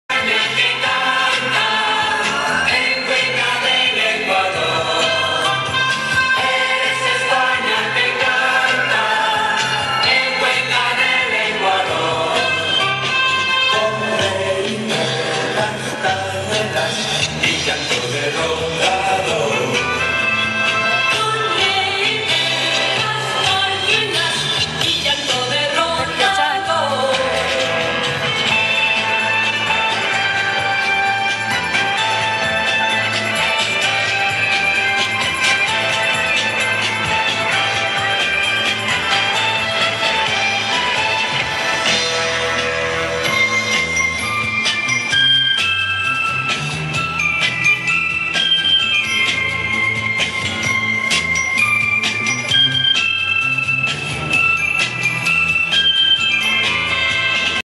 Ecuador Drunk Dancing
Apr 11/12  While waiting for a dance performance (which never materialized) in the Plaza Civica, this fellow took it upon himself to perform and he was really quite entertaining.